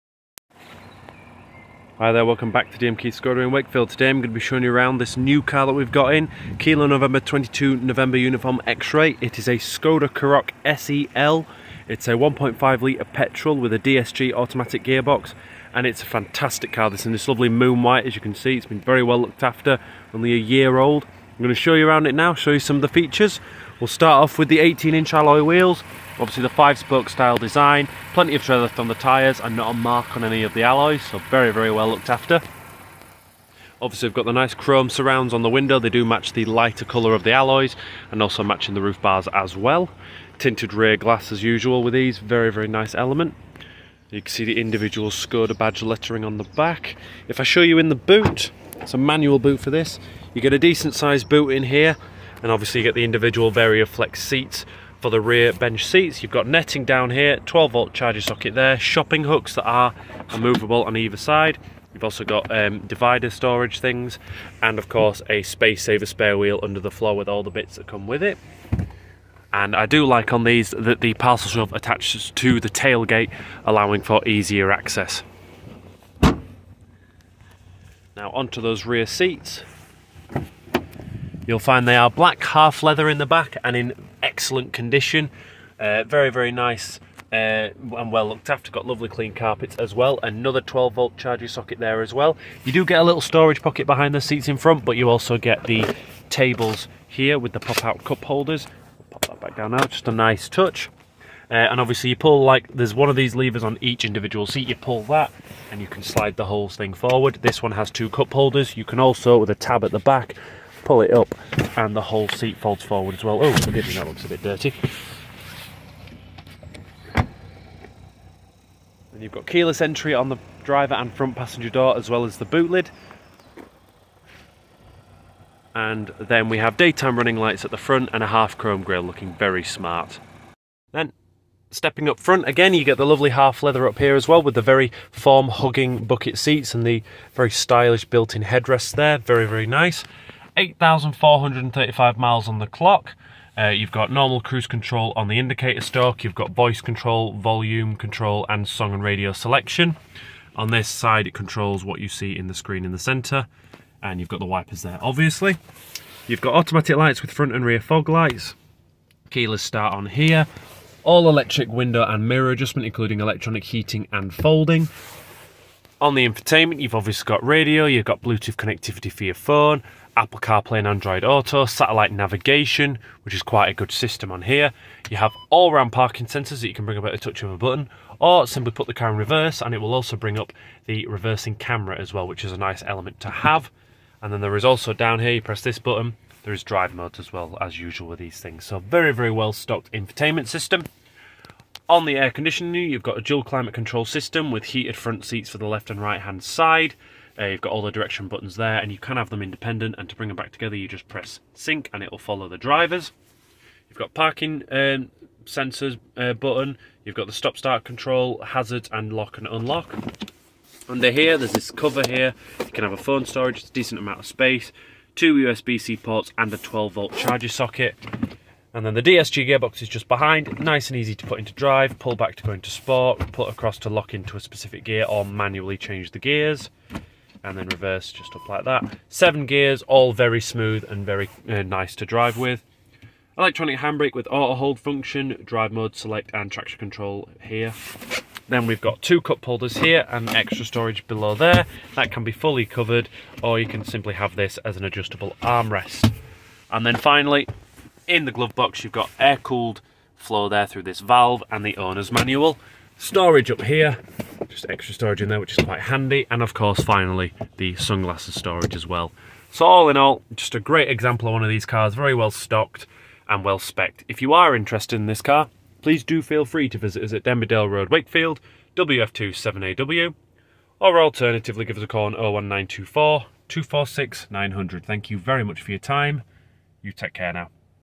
Skoda Karoq 1.5 TSI SE L DSG 5dr
Hello and welcome to DM Keith Skoda Wakefield. Here we are showing you around this lovely Skoda Karoq 1.5 SE L DSG 5dr (KN22 NUX) which we have in stock for you today.

If you are interested in this car and would like to get in touch, please call , or alternatively if you would like to visit us, you can find us at Calder Island Way, Denby Dale Road, Wakefield, WF2 7AW.

Thank you very much, and we look forward to hearing from you.